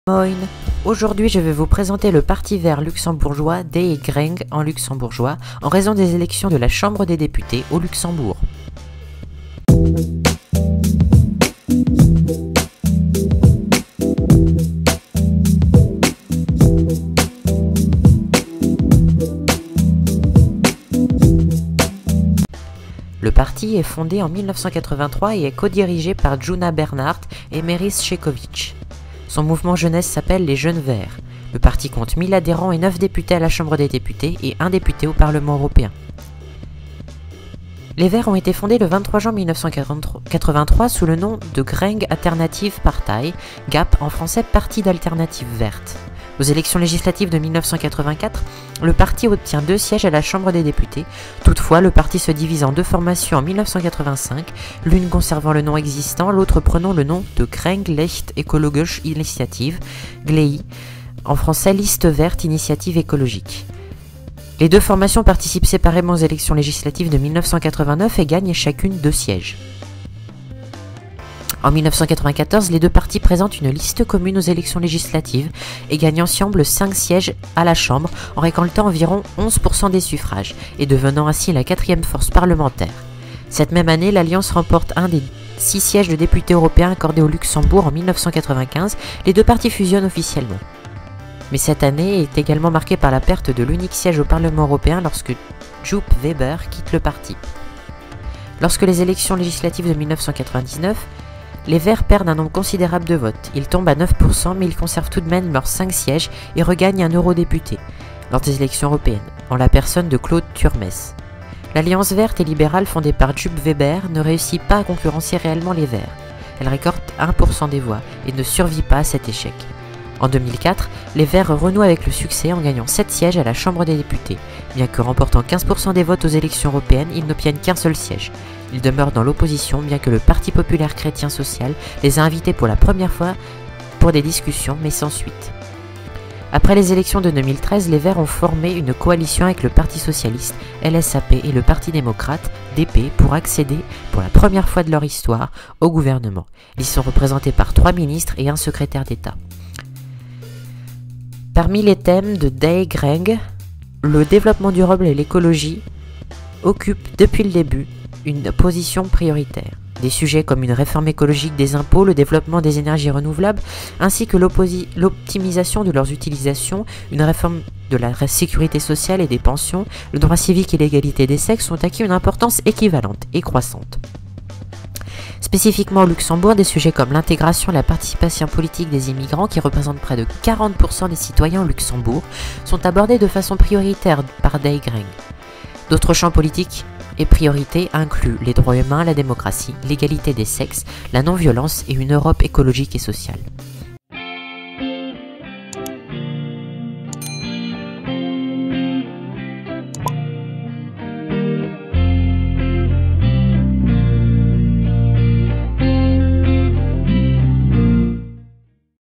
Dans les urnes : déi Gréng (Les verts)
Cette vidéo vous informe sur le parti vert luxembourgeois : déi Gréng.
Mon logiciel de montage (Meine Montage-Software) : Shotcut